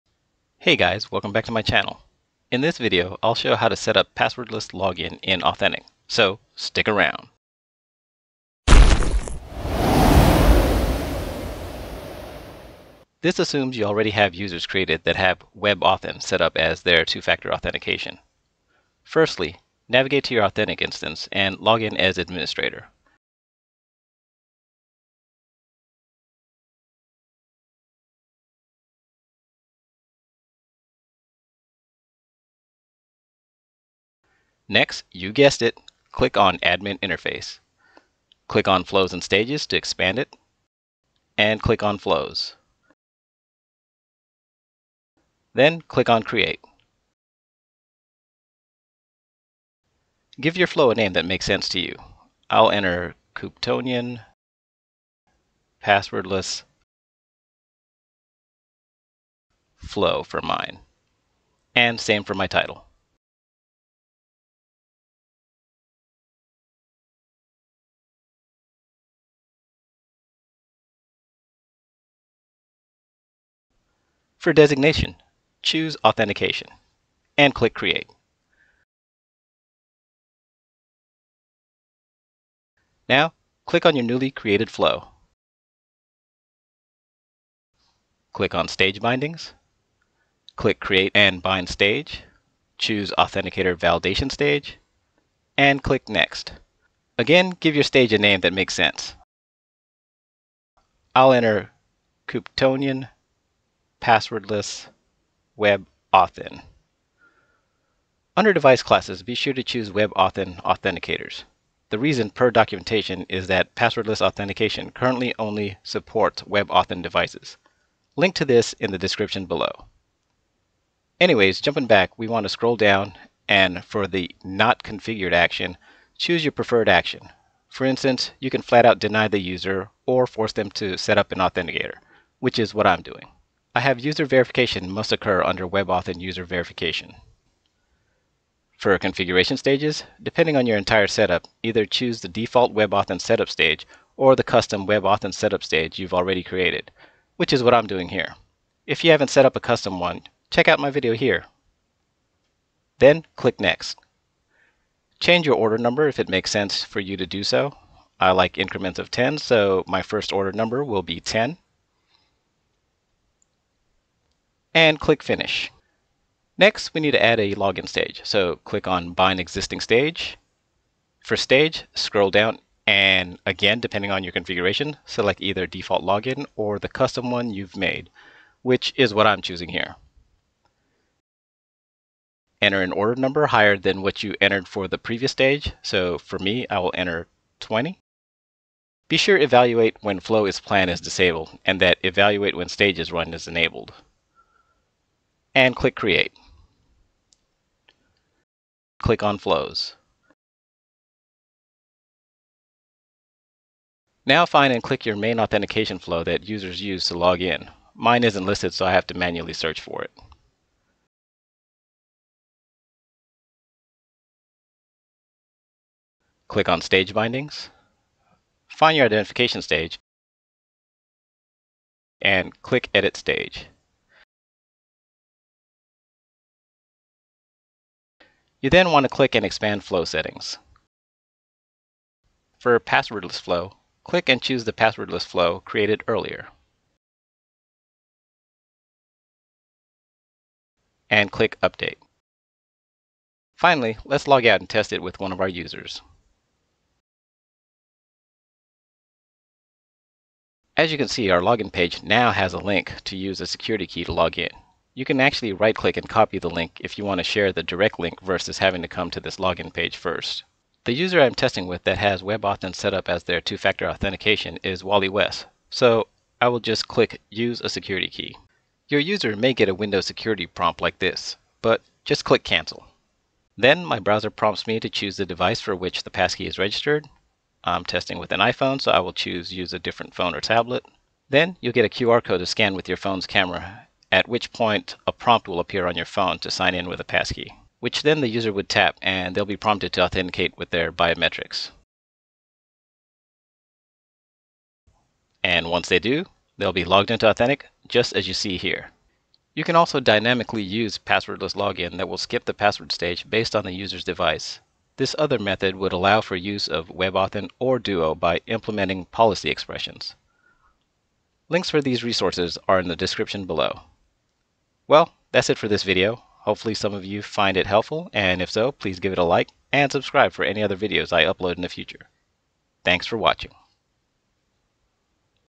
Authentik - Passwordless Login
In this video, I walk through how to set up passwordless login in authentik.